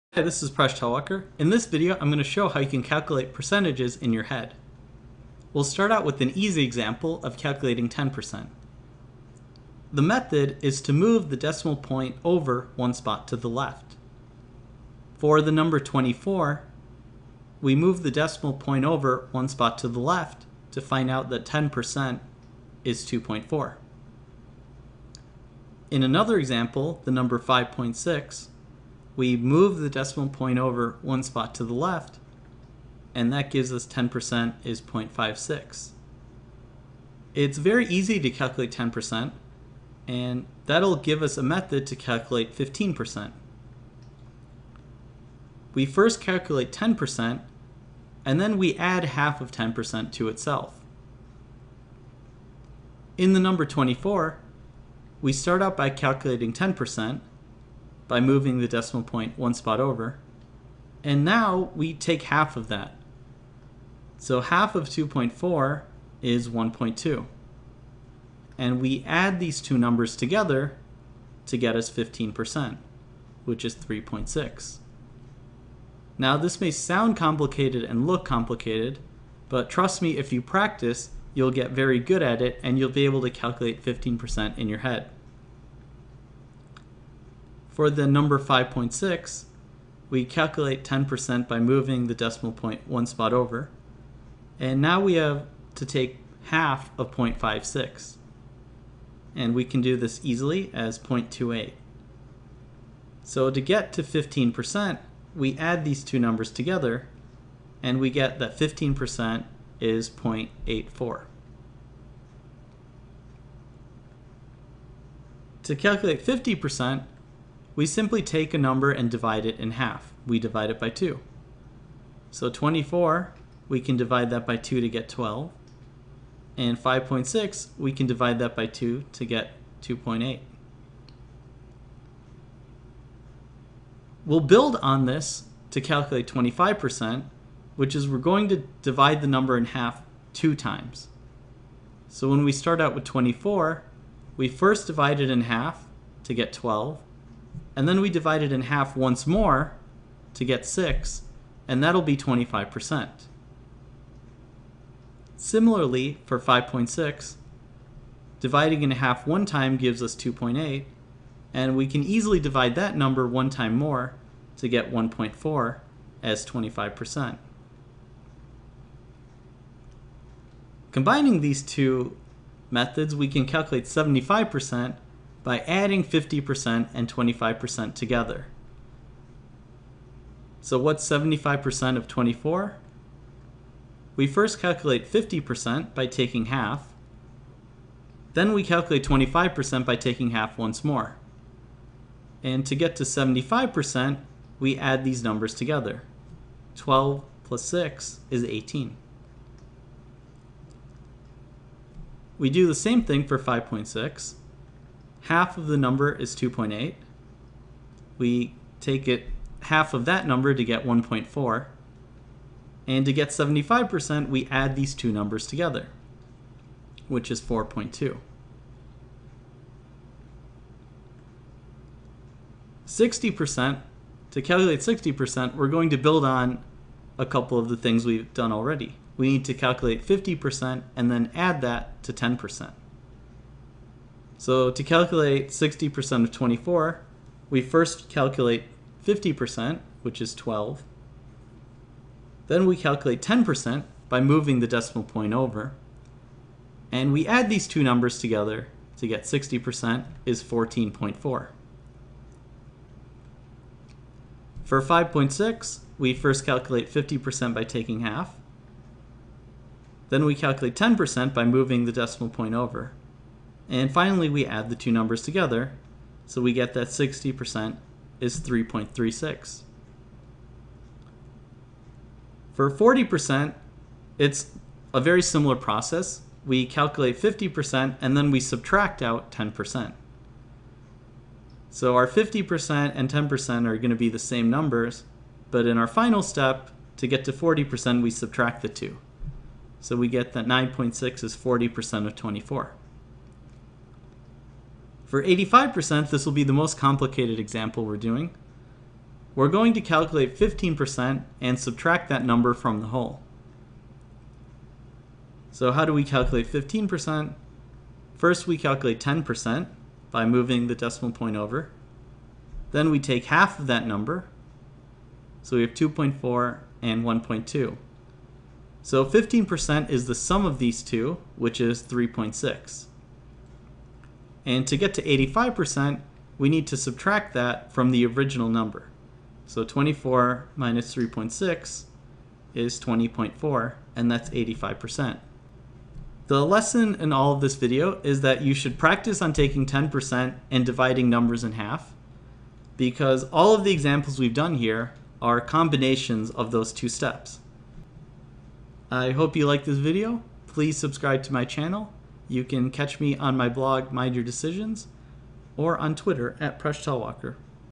How to calculate percentages in your head
Can you calculate 85 percent of 24 in your head? This is not as hard as it sounds. If you know how to halve numbers and divide by 10 you can probably do this in your head. This video explains how to calculate many percentages as a combination of ones that are easier to compute.

My Books